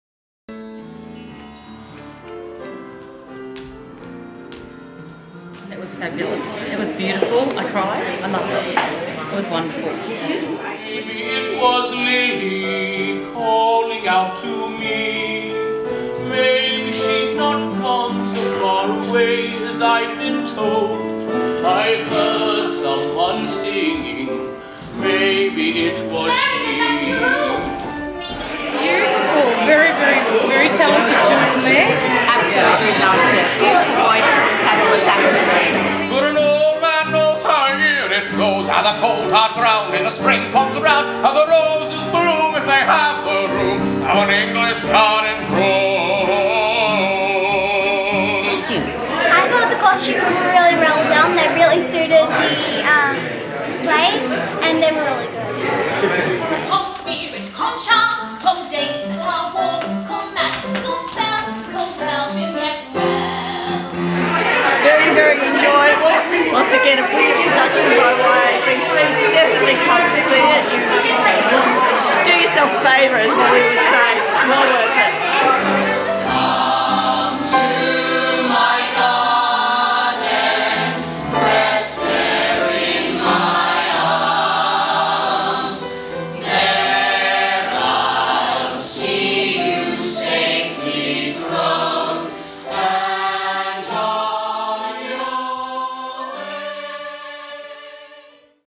Secret Garden - Vox Pops
LATEST NEWS:  Rave reviews from the general public about the Young Australian Broadway Chorus production of Secret Garden - Spring Version.

Book & Lyrics by Marsha Norman
Based on the novel by Frances Hodgson Burnett
By arrangement with ORiGiN™ Theatrical on behalf of Samuel French Inc.